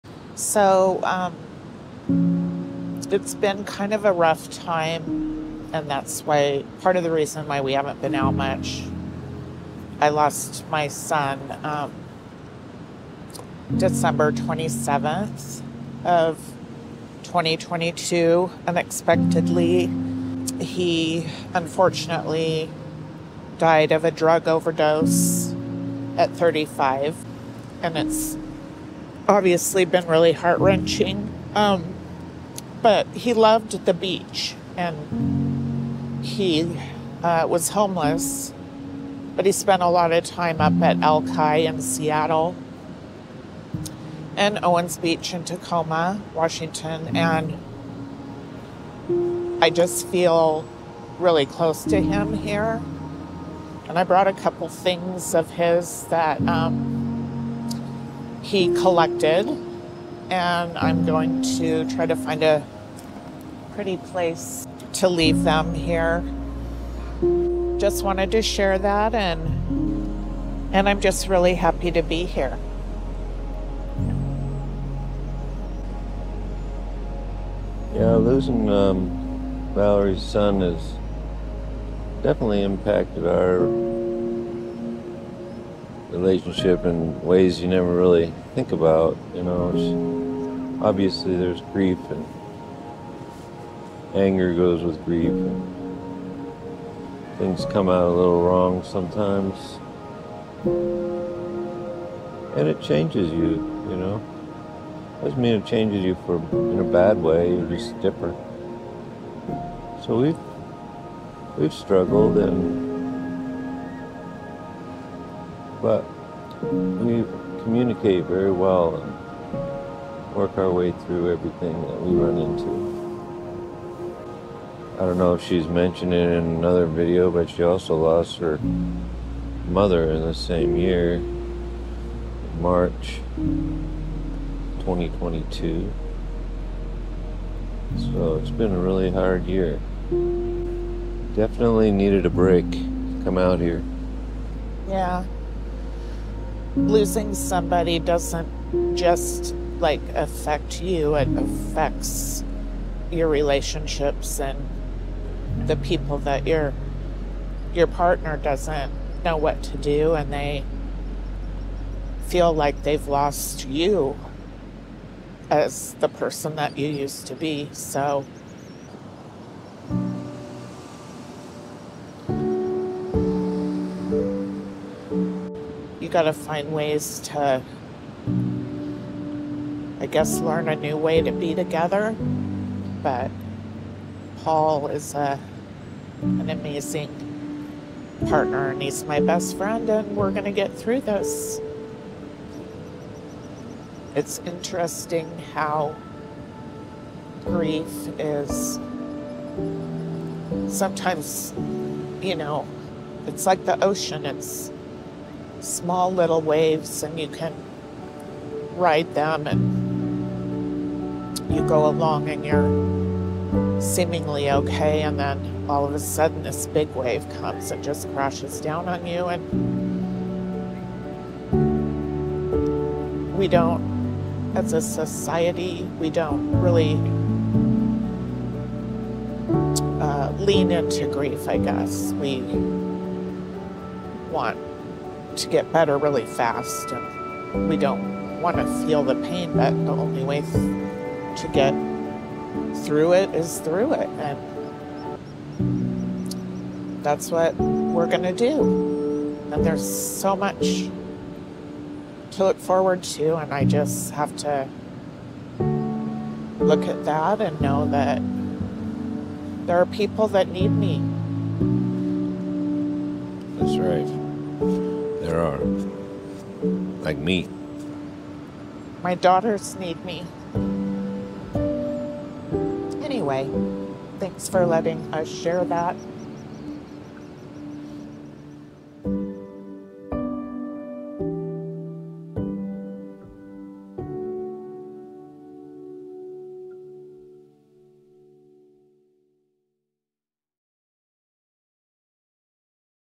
Grief - The loss of a son. Sharing a painful view of our lives this past year, 2022.
Valerie and I felt we could share our honest grief of the loss of her son on Dec. 27th 2022. Little did we know that Christmas day would be the last time on this earth we'd be blessed with his embrace. Until we meet again Shawn.